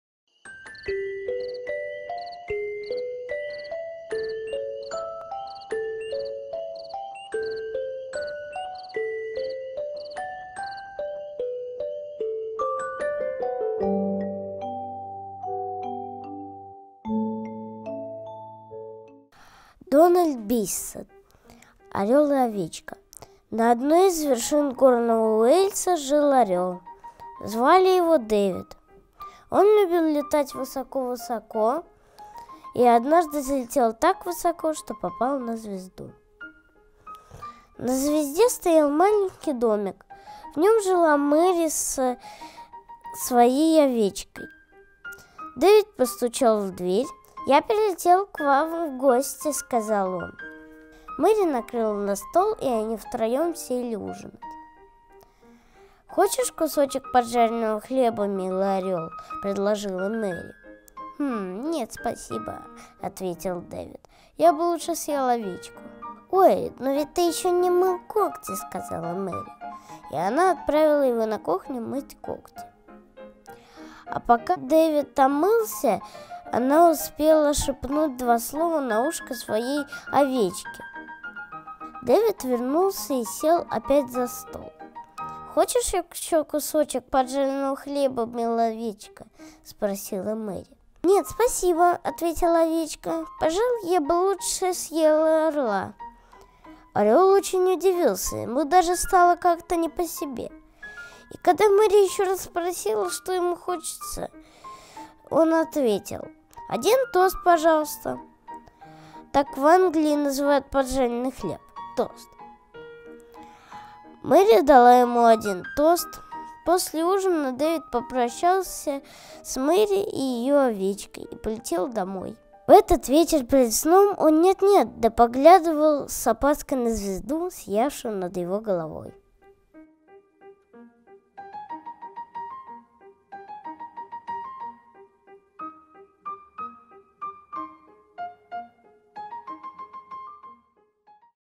Матвей Красник читает сказку Дональда Биссета "Орел и овечка"
Матвей - сын петербургской детской писательницы Ирины Зартайской. Съемка для проекта "Читаем детям" его первый опыт публичного выступления, и справился он отлично.